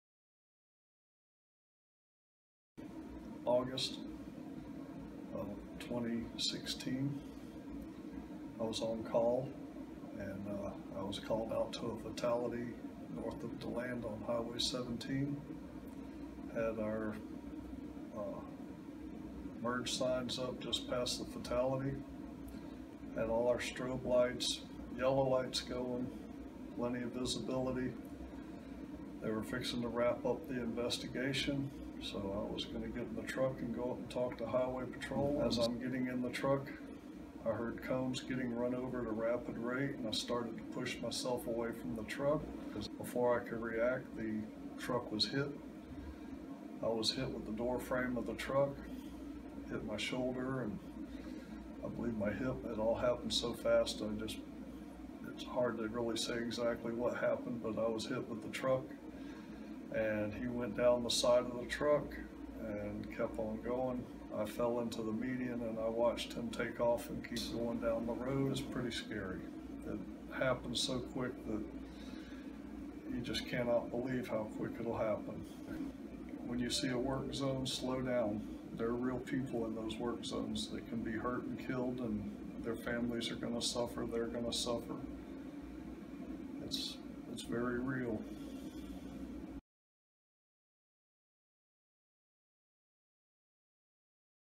Kenny Talks About Being Involved in a Work Zone Crash
Kenny works at our Kepler Operations Office. He recalls a scary day from August 2016 where he was involved in a work zone crash.